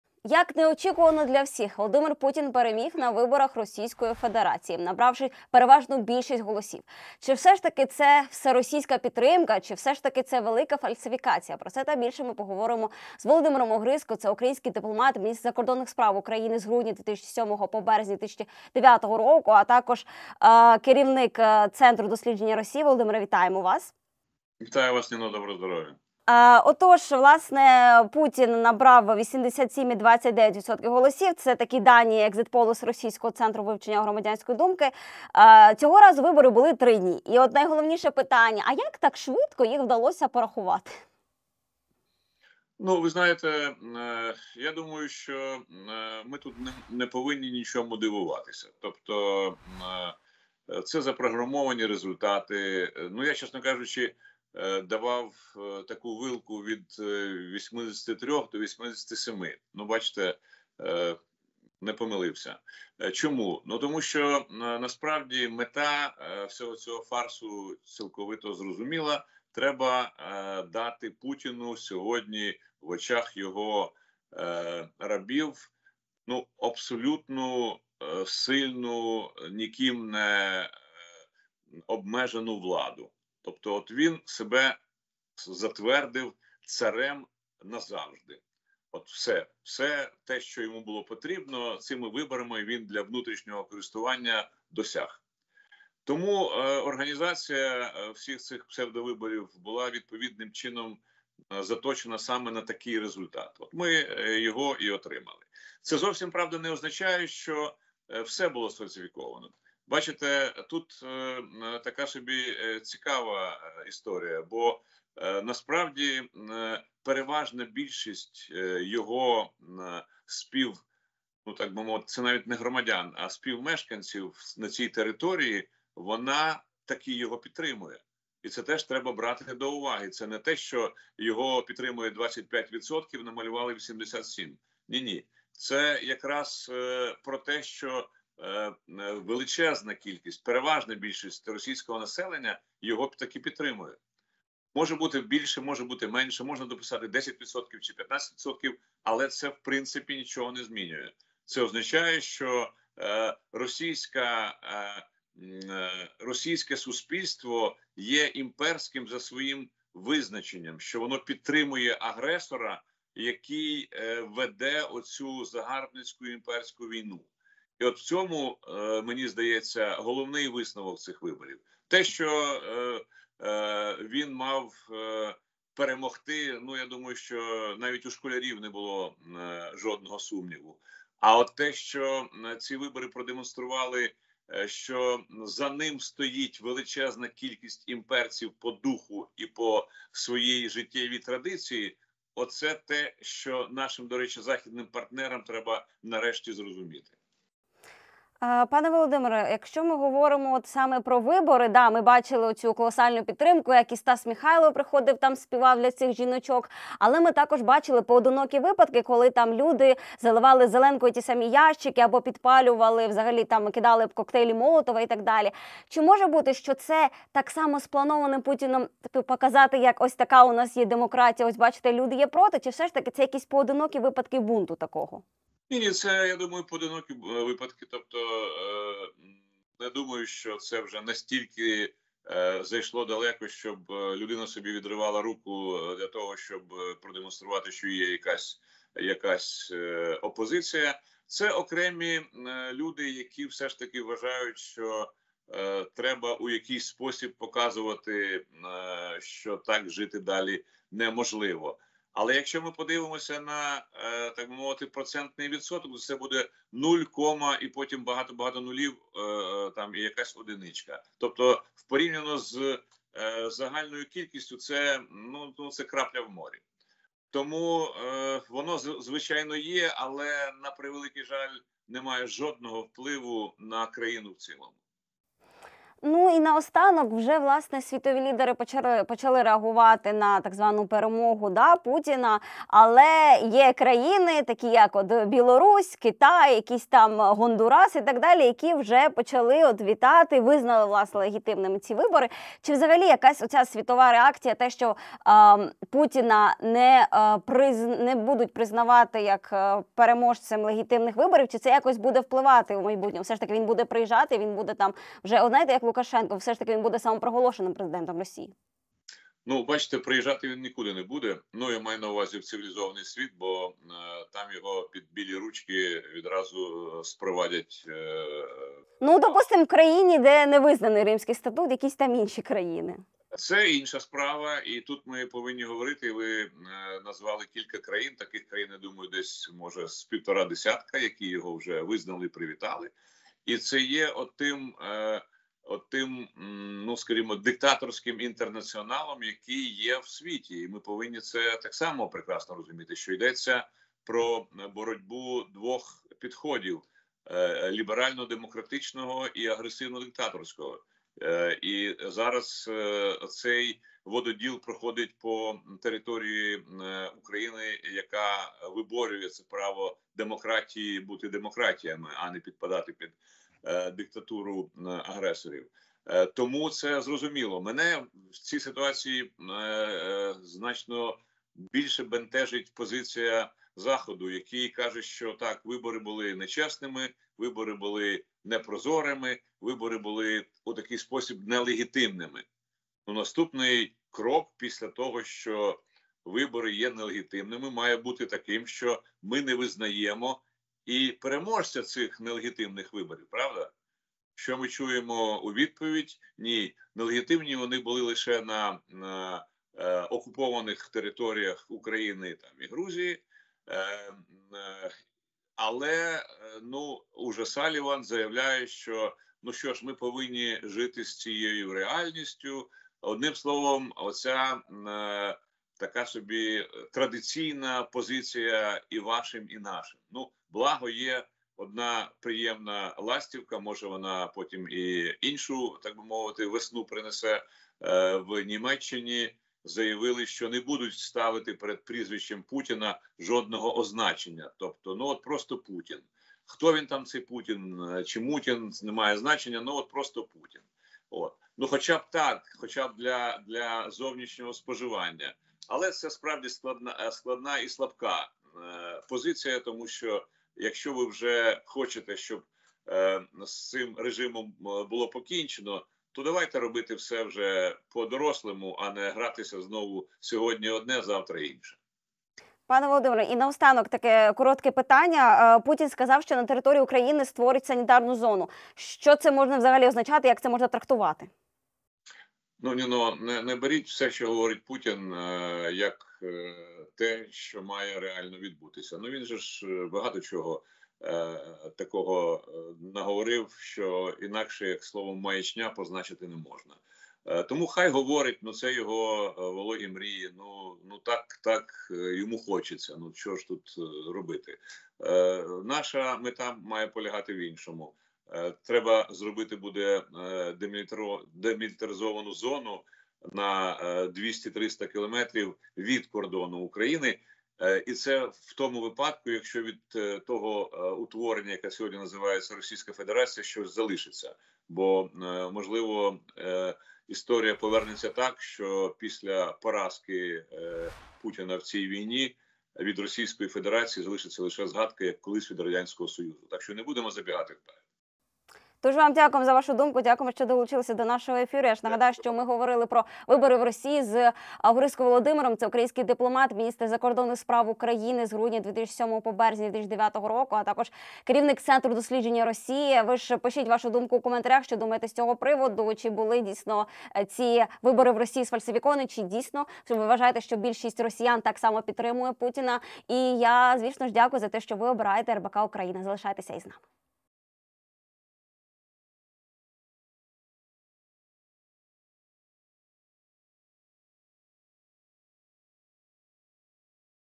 «Світ визнав вибори Путіна незаконними, але нічого з цим не робить” - Володимир Огризко
Захід визнає вибори в Росії незаконними, але залишається байдужим. Про це в ефірі РБК-Україна заявив Володимир Огризко, український дипломат, міністр закордонних справ України з грудня 2007-го по березень 2009 року, керівник Центру дослідження Росії. “Мене бентежить позиція Заходу, який каже, що вибори були нечесними, непрозорими і нелегітимними. І наступний крок має бути невизнання переможця цих виборів. А у відповідь ми чуємо: “Ні, нелегітимні були вони лише на окупованих територіях України та Грузії”. Салліван заявляє, що ми повинні жити з цією реальністю. Це традиційна позиція «і вашим, і нашим». Але є одна приємна новина, у Німеччині заявили, що не будуть ставити перед прізвищем Путіна жодного означення. Просто Путін. Хоча б так. Але це слабка позиція, бо якщо ви хочете, щоб з цим режимом було покінчено, то давайте робити все по-дорослому, а не гратися сьогодні в одне, а завтра в інше” - заявив український дипломат Володимир Огризко.